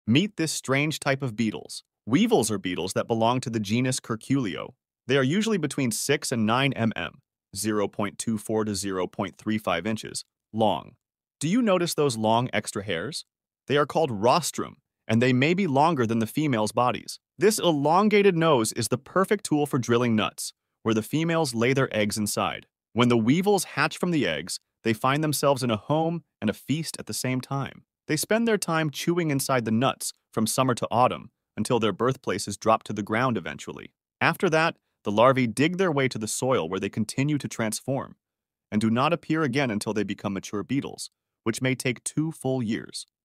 The Weevil: A Tiny Beetle with a Big Impact
Strange but fascinating! Weevils are a type of beetle with long noses called rostrums, which they use to dig up nuts to lay their eggs in. The larvae hatch inside the nuts and feed on them until they fall to the ground. The larvae then burrow into the soil and continue to develop for up to two years before emerging as adult beetles.